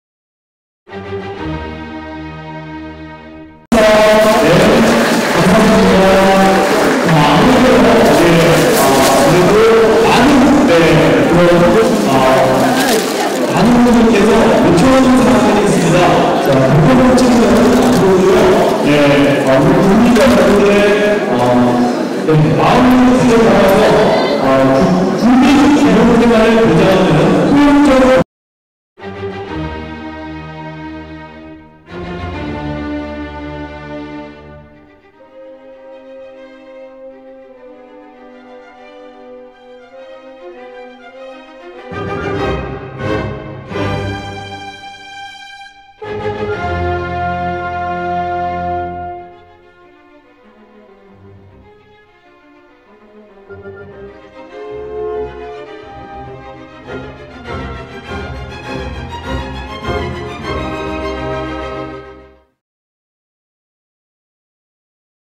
[논평] '장애인 비하 사과하라' 황교안 씨 향한 기습 시위…"번개불에 콩구워먹은 기쁘고 즐거운 희망에 찬 기습시위 여성들 대단합니다. 아름답습니다. 축복합니다" 국제언론인회
장애인차별철폐연대 등 장애인단체 활동가들이 6일 오전 서울 여의도 63빌딩 컨벤션센터에서 열린 사회복지의 날 기념식에 참가한 황교안 자유한국당 대표를 향해 "장애인 비하·차별 발언 사과하라"는 기습 시위를 벌이고 있다.